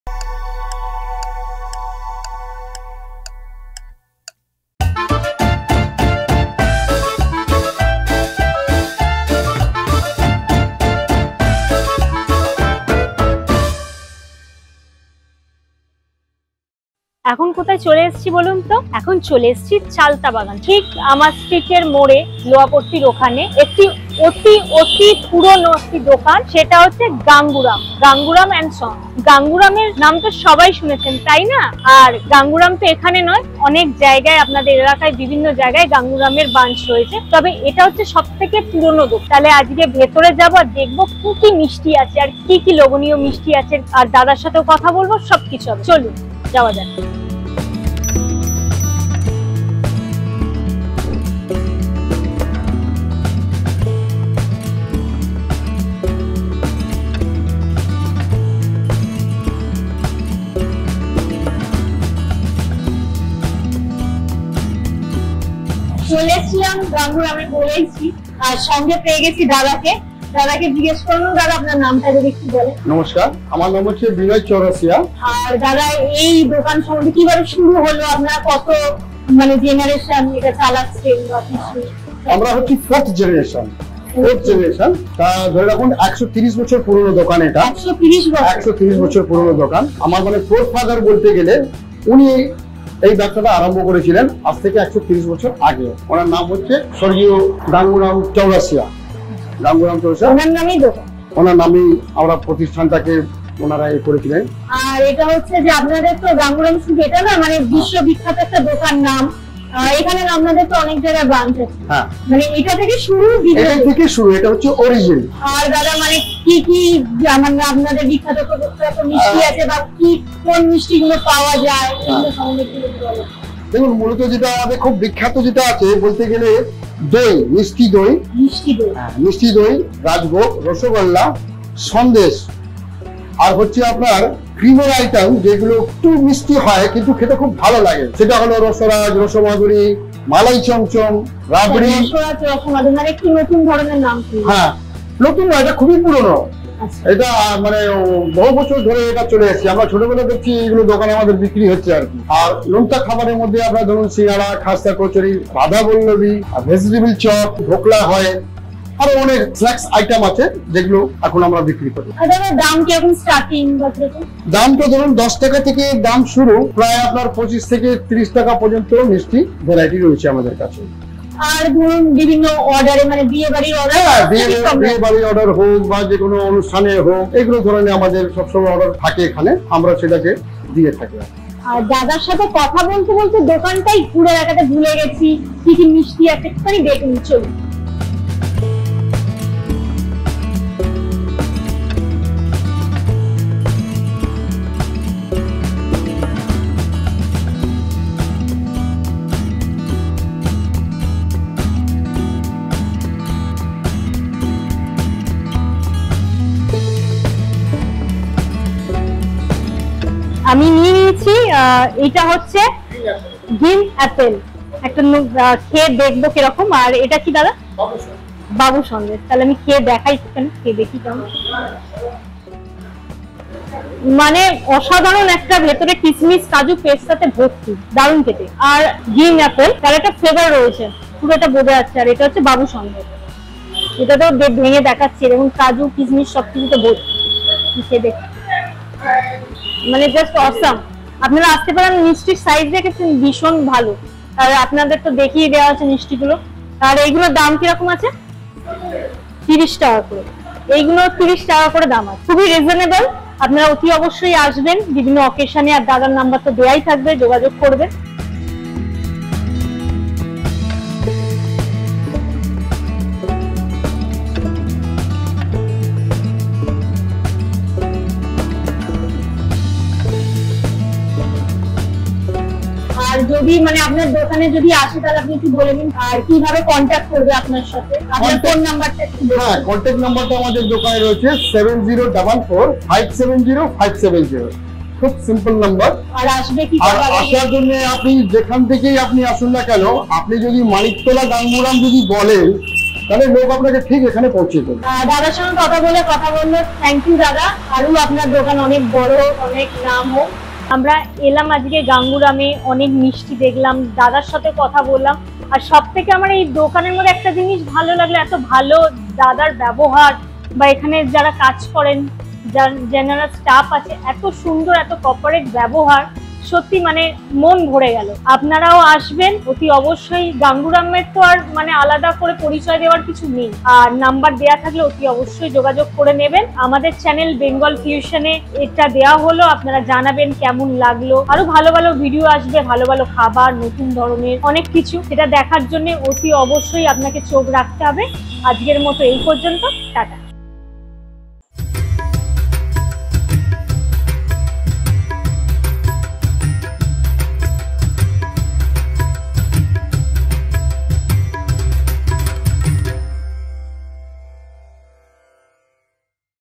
Ganguram & Sons | 130 Years Famous Sweet Shop | অভিনব মিষ্টি না খেলেই মিস্‌। | Maniktala | Kolkata
Ganguram and Sons
Address: 159C, Vivekananda Rd, Manicktala,
Azad Hind Bag, Kolkata, West Bengal 700006

যে কোনোরকম ব্যবসায়িক প্রয়োজনে বা চ্যানেলের উন্নতির জন্য পরামর্শ জানাতে সরাসরি কথা বলুন BENGAL FUSION এর এই ফোন নম্বরে অথবা ইমেল করুন।